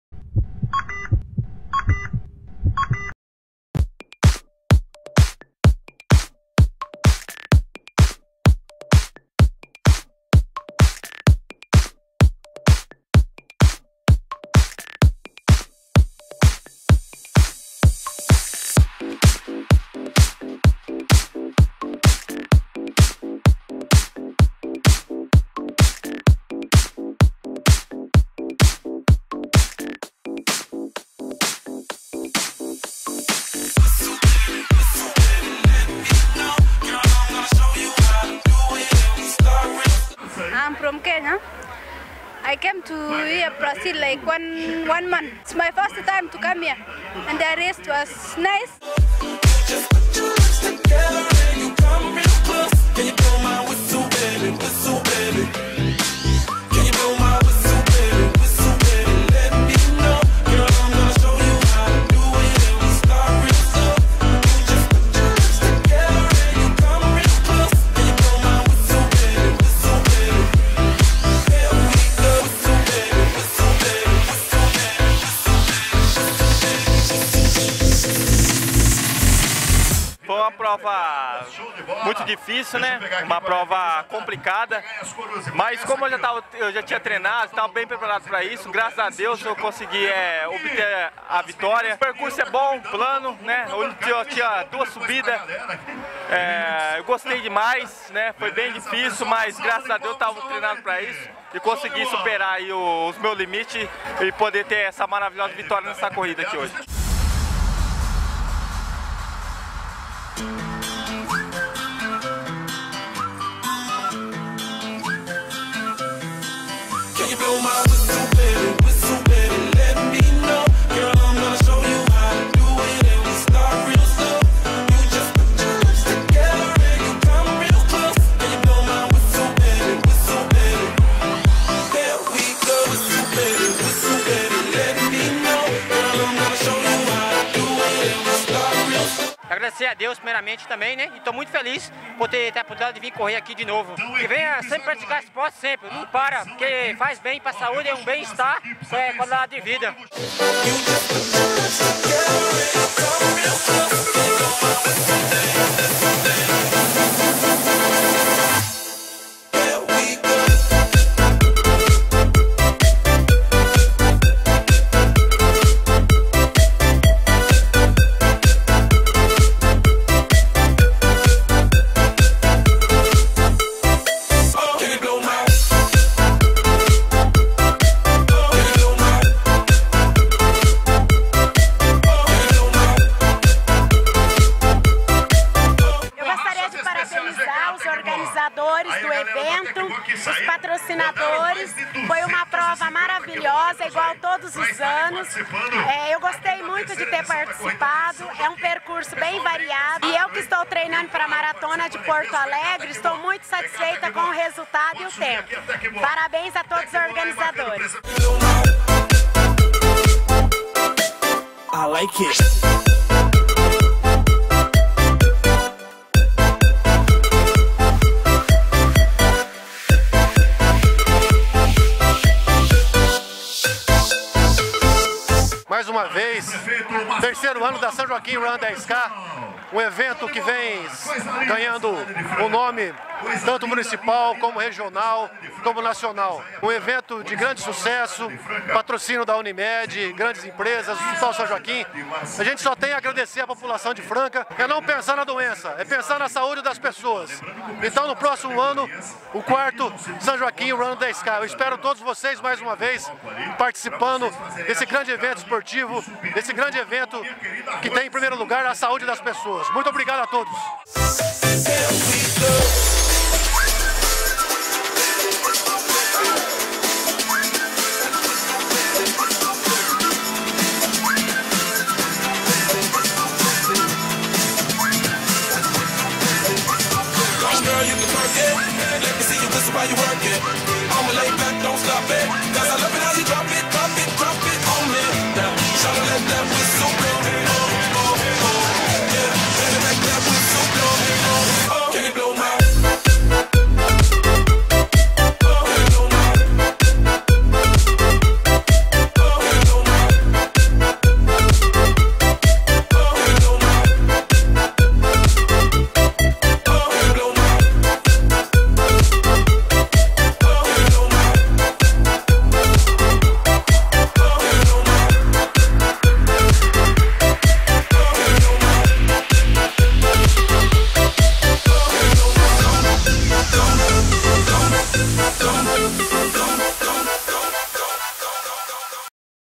III São Joaquim Run
Ontem (21/4) aconteceu na cidade de Franca/SP a terceira edição da São Joaquim Run. Um evento que enaltece a qualidade de vida e a saúde das pessoas. Confira tudo que aconteceu neste vídeo exclusivo!